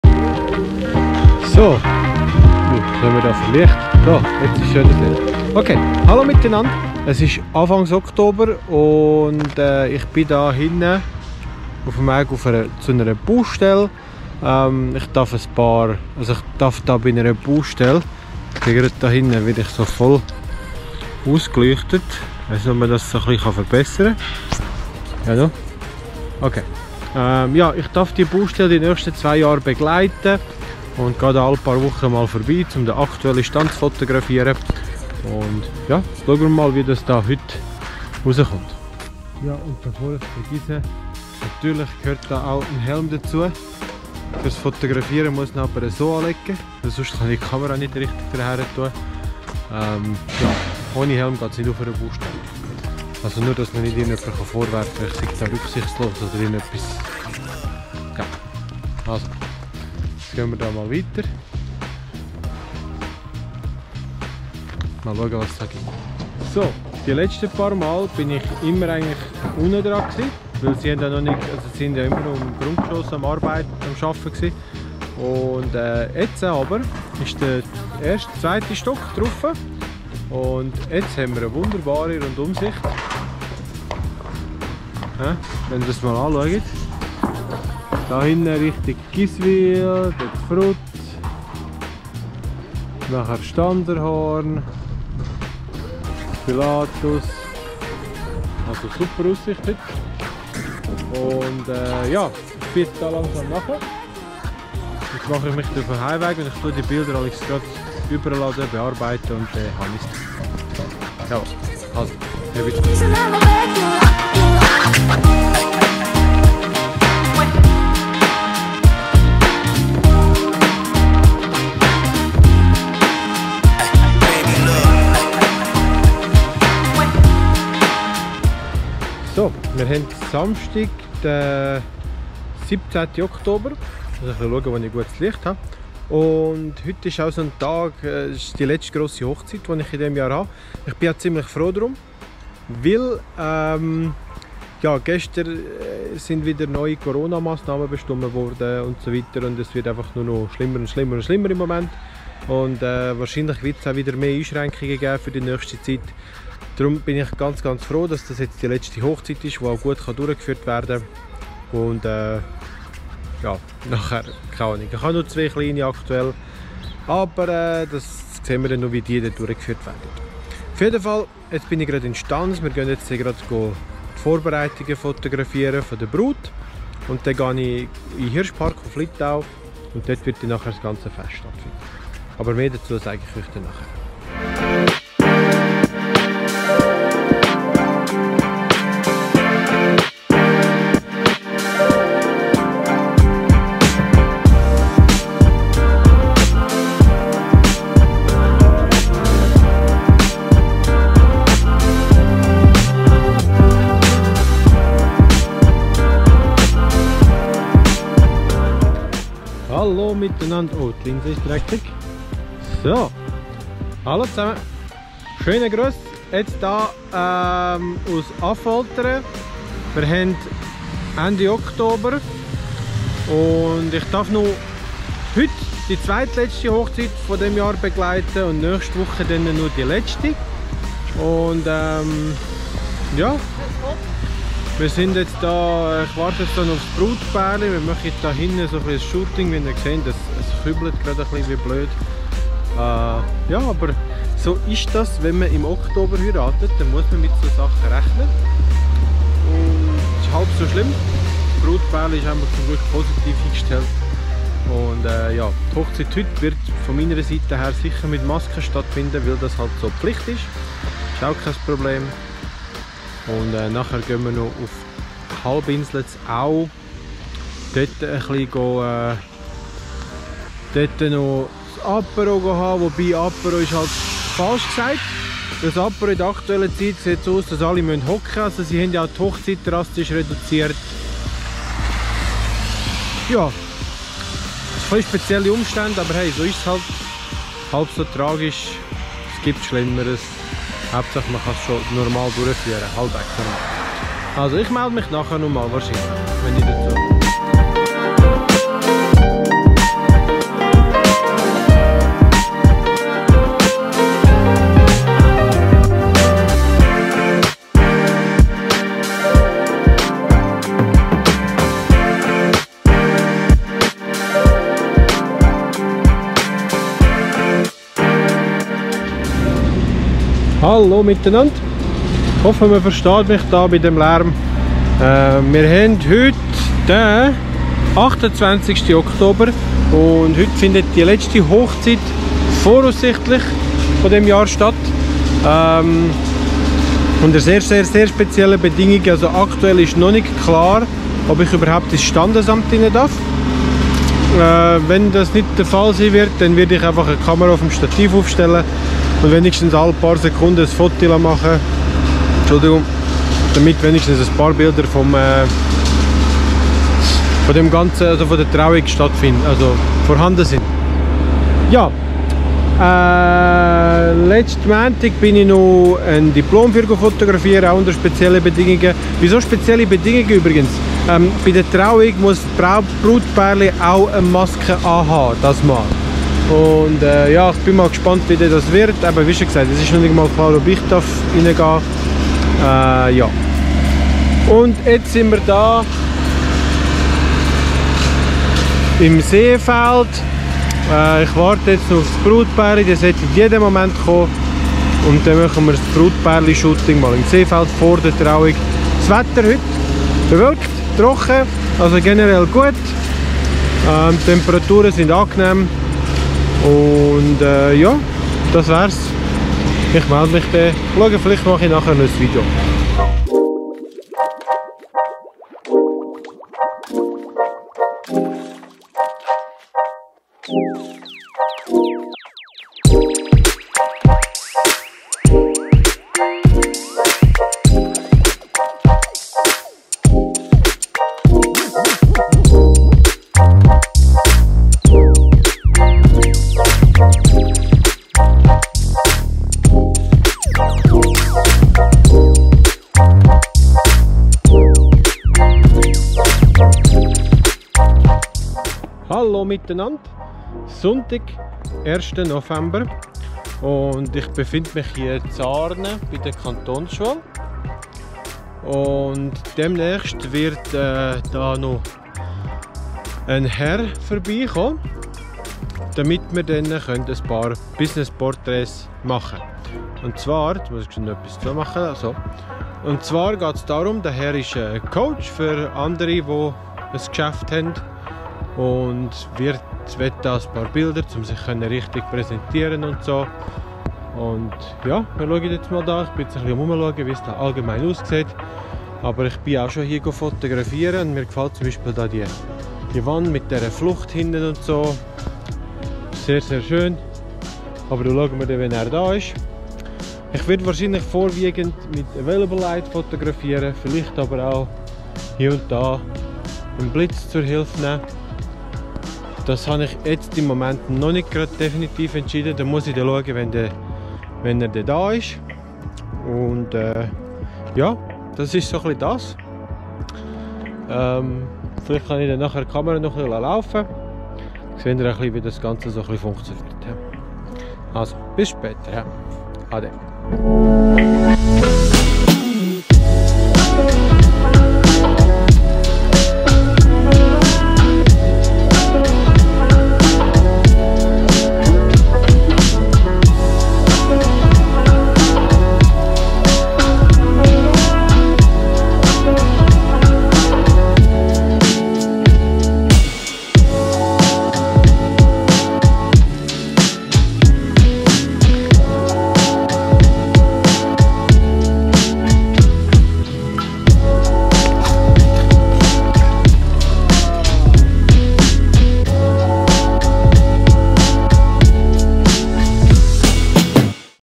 VLOG Oktober 2020 - Izedin Fotografie
Ein kurzer VLOG Vom Oktober 2020. Ich konnte leider nicht alle Aufträge hineinnehmen, gebe mir aber Mühe ,dass das bei den nächsten VLOGS wieder besser wird :-)

Gefallen dir diese Einblicke? Lass es mich gerne wissen!